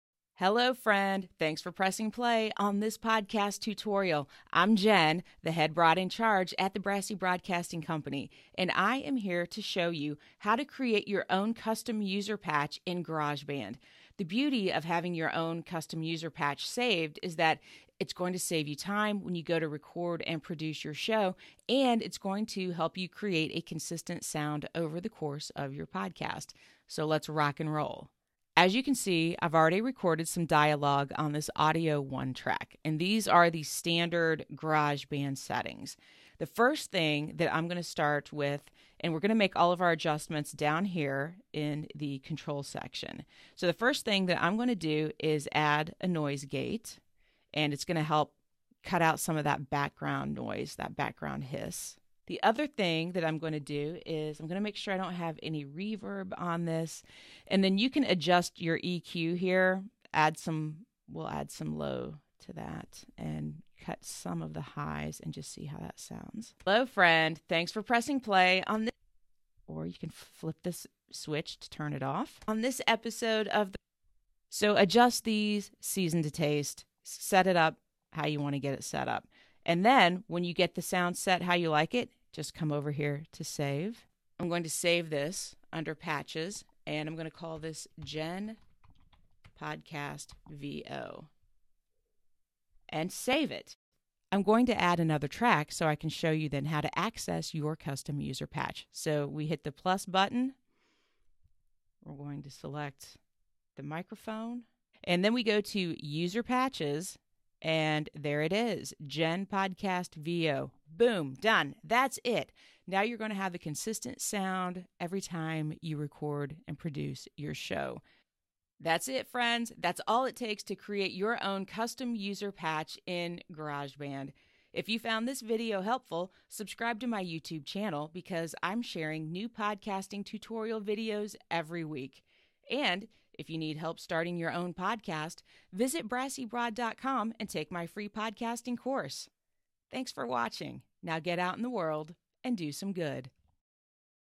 How to Make Your Own Podcast Voice Setting in GarageBand
Making your own custom podcast setting for your voice track in GarageBand will make your life so much easier when you go to record and edit your podcast.

This tutorial was recorded using the Audio Technica ATR2100 microphone plugged directly into a MacBook Pro.

The Brass Tacks Free Podcasting Course